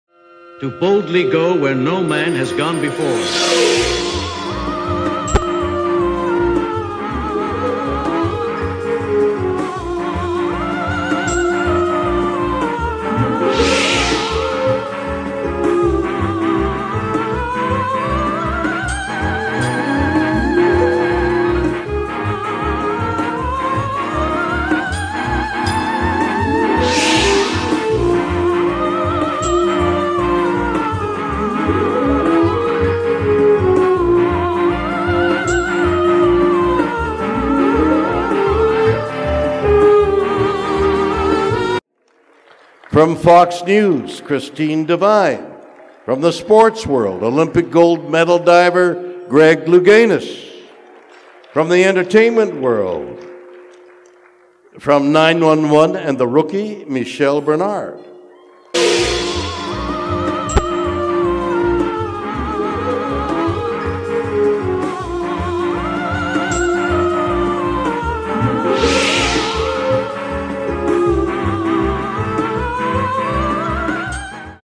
When William "Captain Kirk" Shatner said my name....
At the jackwebb awards gala, hosted by williamshatner for the @losangelespolicemuseum2272, after being raised a "Trekkie," to say I was beyond humbled and grateful, not to mentioned shocked, is truly an understatement...
Thank you so much for all of the continued love and support, I am full of gratitude!
Repost: I was raised a trekkie, hard core, sci-fi nerd all the way! 💫 Star Trek, both the tv series and movies, were a big part of my childhood and wonderful memories of wanting to be a star in Hollywood.
Last weekend, I was invited to attend the Los Angeles Police Museum annual jackwebbawards gala, hosted by THE CaptainKirk, William Shatner! At the beginning of the ceremony, Bill was introducing all of the honorees, nominees, benefactors, special guests, and celebrities in attendance, and last, but certainly not least, was l'il ol' me! I died! Omfg Captain Kirk said my name!!! Wtf!!! Seriously? 🤪 I was in such shock, that my new friends at the dinner table all said, "stand up, get up!" I did. I smiled, waved, gave thanks, and then sat back down with a flushed face, still not believing that WilliamShatner gave me a shout-out and said my name!! (Footage is coming soon...)
So after the ceremony, and we all sat down to eat dinner, I walked over to Bill's table and introduced myself, he said, "Oh, I just mentioned you, sit down with me." So, I sat next to him. We chatted for almost 20 minutes. I told him about my modeling booking and meetings with Leonard Nimoy in 2007, sharing the personal stories and journey that Leonard shared with me about his career. Bill smiled, taken aback I knew the whole story and remembered so many details. I mentioned Leonard's love for Bill, not just in the show, but the brotherhood they shared the rest of their lives. I mentioned that I knew they were only 4 days apart, born the same year, 1931, William on 3/22 and Leonard on 3/26, and they booked StarTrek when they were 35. Again, he was floored I knew all of this. Then I joked and said, "the man of my dreams is also an Aries and a Canadian," he laughed and said, "well, good luck with that one!" 🤣 Bill told me about his new book and how he celebrated his 91st bday in space and it was magical. 🧚‍♂️ I told him I was incredibly humbled he had taken the time to speak with me for so long and asked for a photo, he said it was his honor. 🙏 A true gentleman and gracious peer. It was a moment I will never, ever forget. 🖖 livelongandprosper my friends... 😊
HAPPY THANK YOU MORE PLEASE... ❤